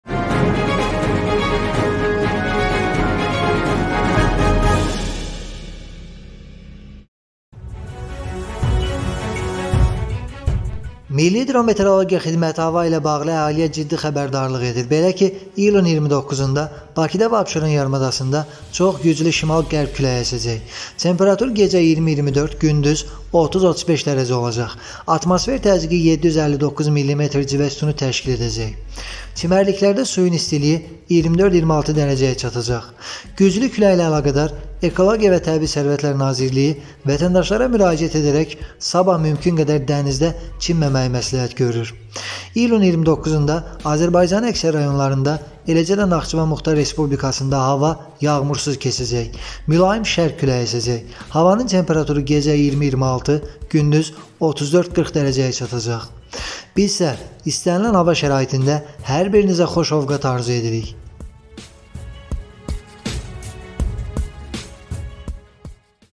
Hava ilə bağlı CİDDİ XƏBƏRDARLIQ: Sabah gün ərzində...
Hava ilə bağlı xəbərdarlıq edilir. Sabah Bakı və abşeronda yarımadasında güclü şimal qərb küləyi əsəcək.